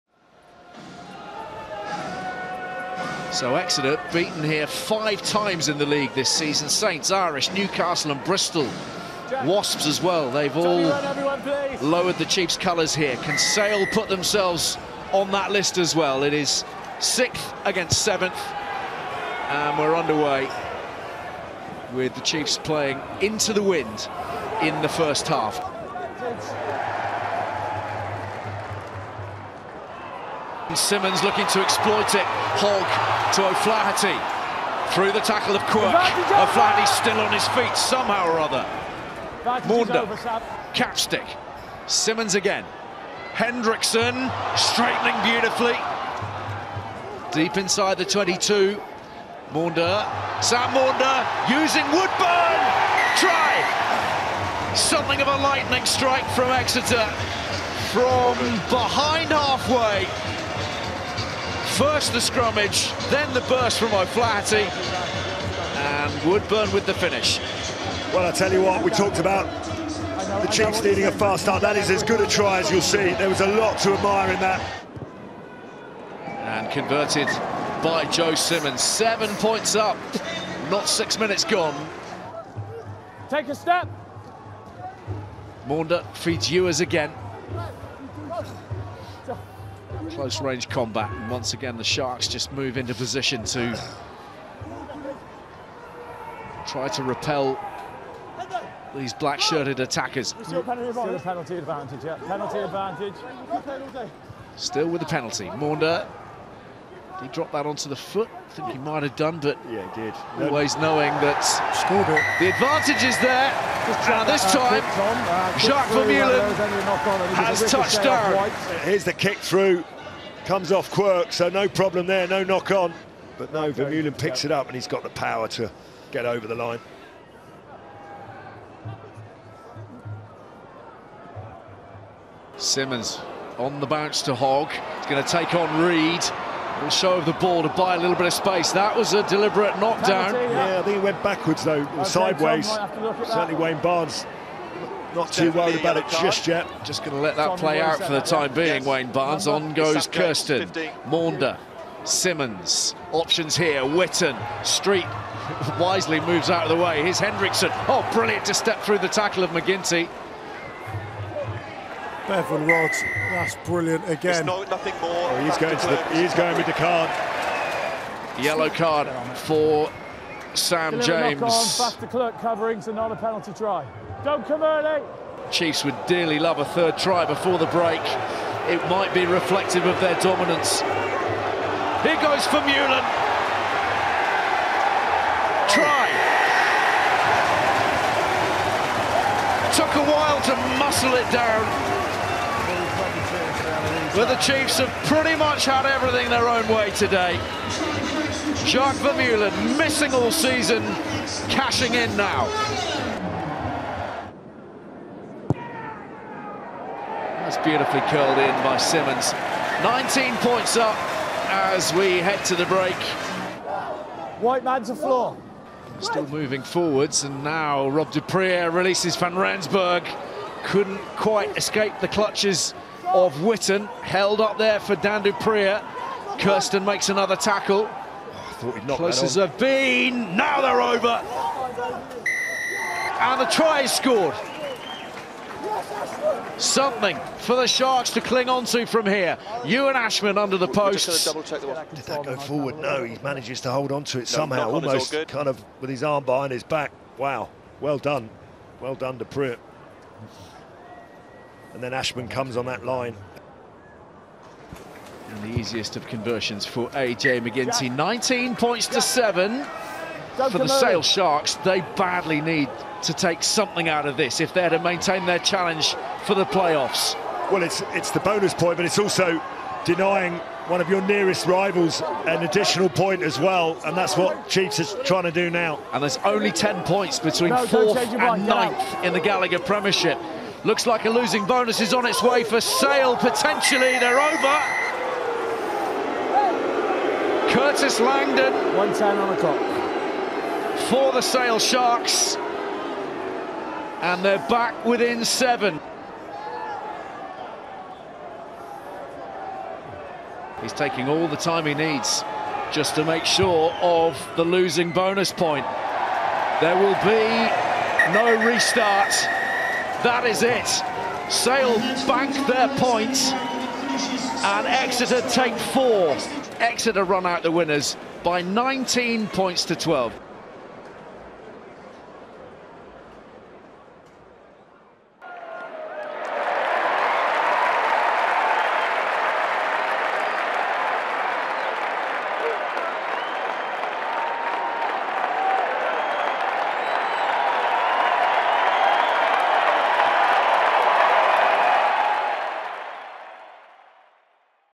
Exeter v Sale - HIGHLIGHTS | Vermeulen's Back with a Bang! | Gallagher Premiership 2021/22
Watch match highlights of Exeter Chiefs vs Sale Sharks from the Gallagher Premiership 2021/22